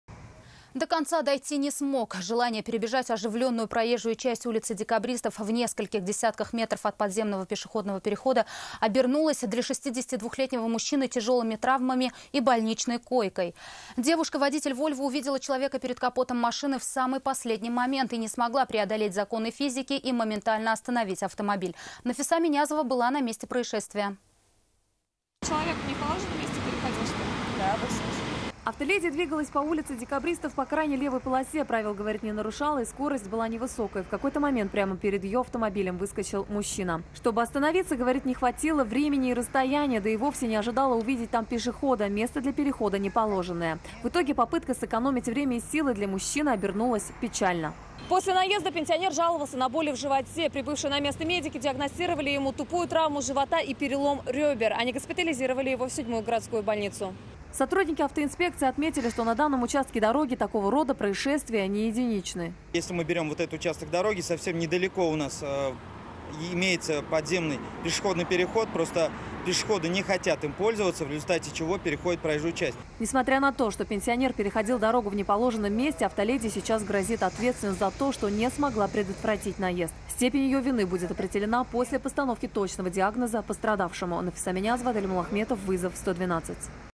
Желание перебежать проезжую часть ул.Декабристов обернулось для мужчины тяжелыми травмами - ТНВ
До конца дойти не смог. Желание перебежать оживленную проезжую часть улицы Декабристов, нескольких десятках метров от подземного пешеходного перехода, обернулось для 62 летнего мужчины тяжелыми травмами и больничной койкой. Девушка - водитель  Volvo, увидела человека перед капотом машины в самый последний момент, и не смогла преодолеть законы физики, и моментально остановить автомобиль.

Координаты:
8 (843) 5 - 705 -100